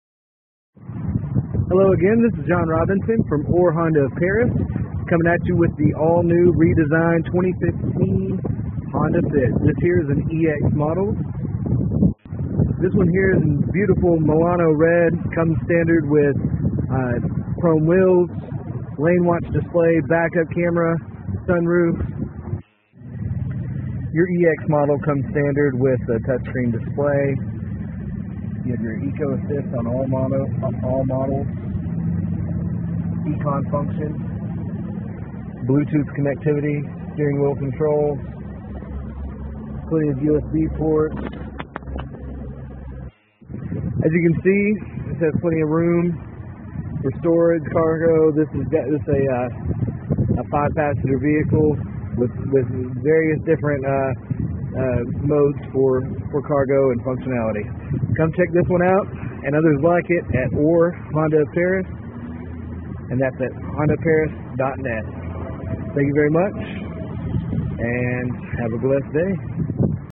2015 Honda Fit EX
2015 Honda Fit EX, with a all new redesign this feature loaded compact car is sure to impress. With lane watch display,Bluetooth, back up camera and more check out this great ride @ hondaofparis.com